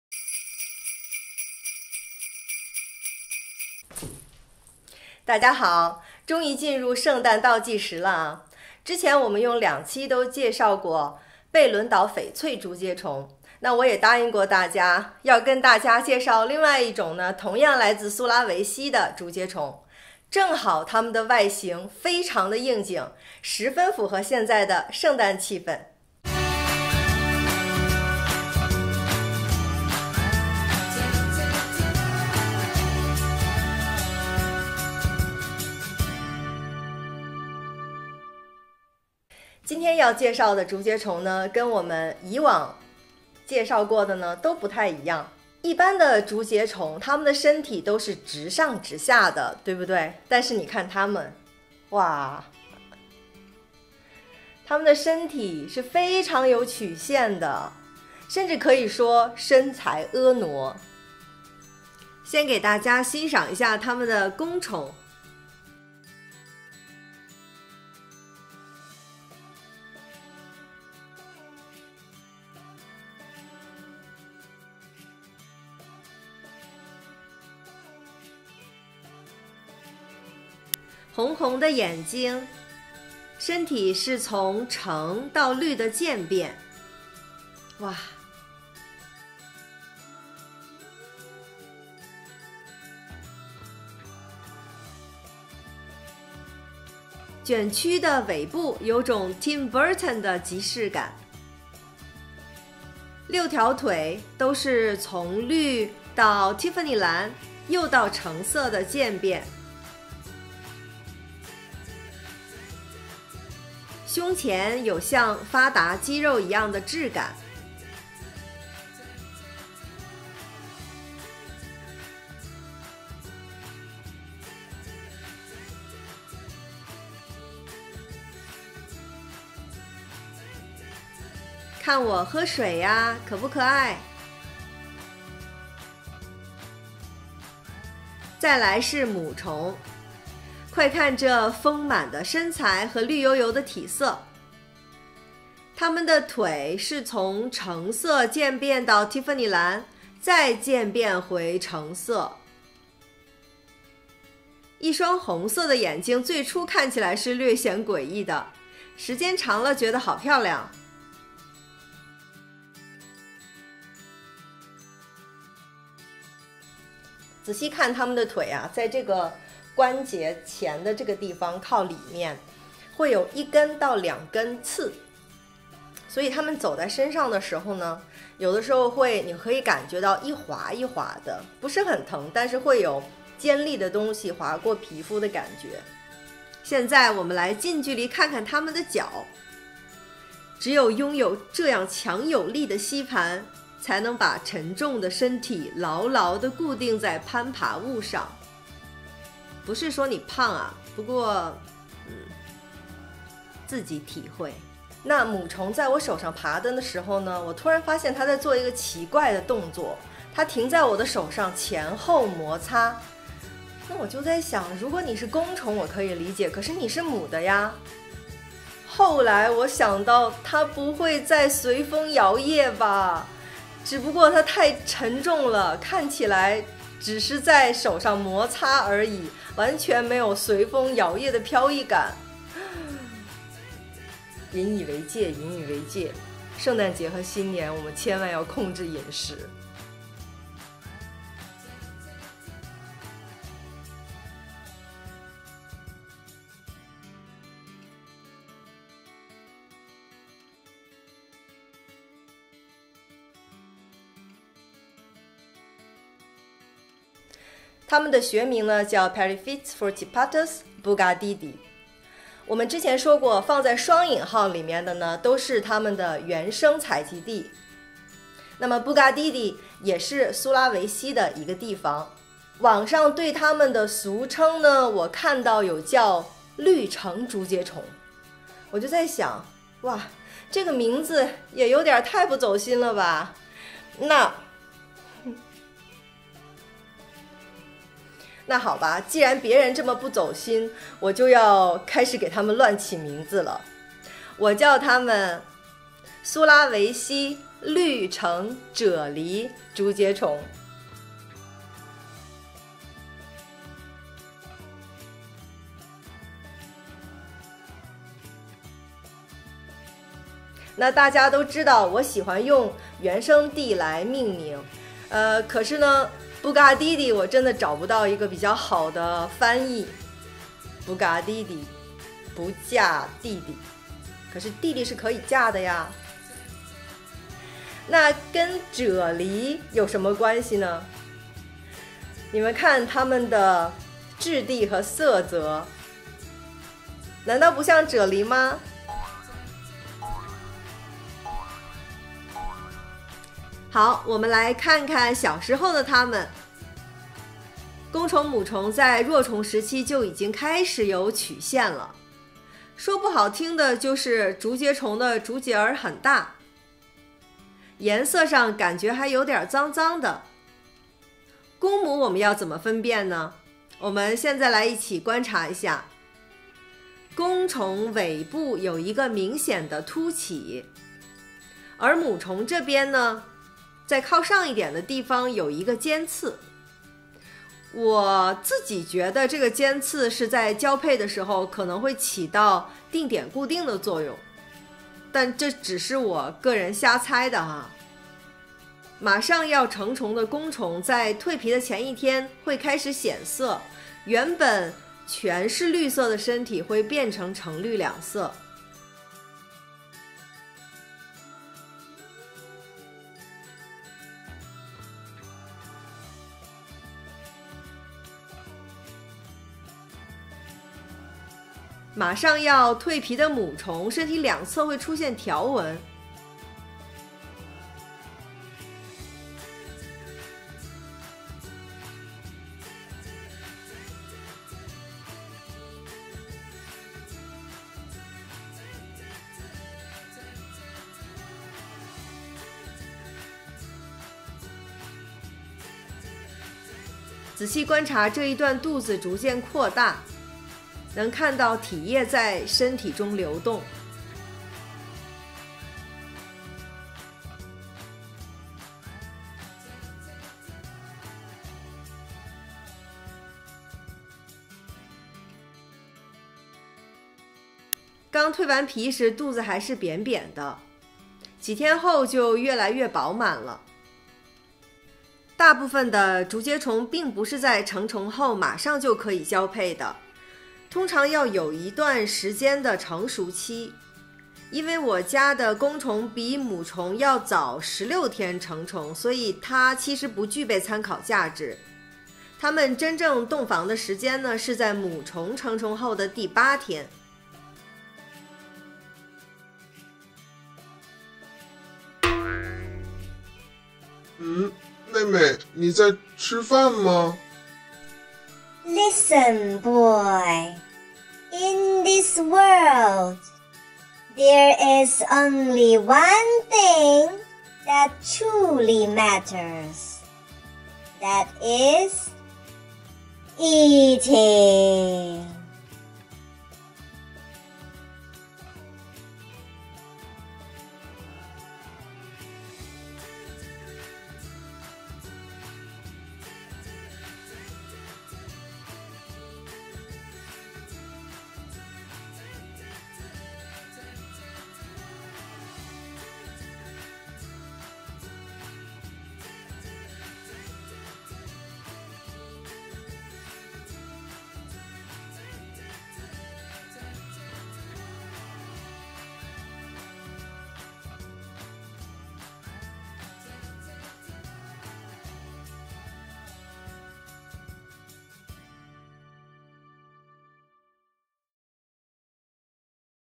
体态婀娜的苏拉威西绿橙啫喱竹节虫－Periphetes forcipatus "Bugadidi" (stick insect, walking stick)
这次介绍来自苏拉威西的竹节虫－Periphetes forcipatus "Bugadidi"，十分符合现在的圣诞气氛